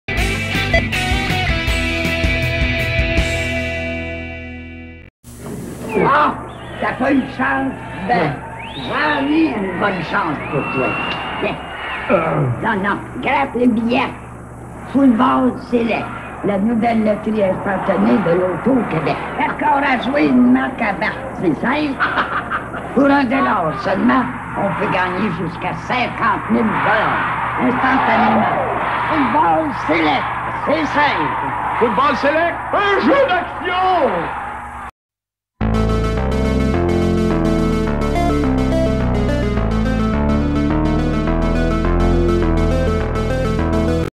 Pub du Jour # 37 Football Select avec Claude Michaud et La Poune 1984
Une publicité de loterie de Football mettant en vedette Claude Michaud et Rose Ouellette La Poune...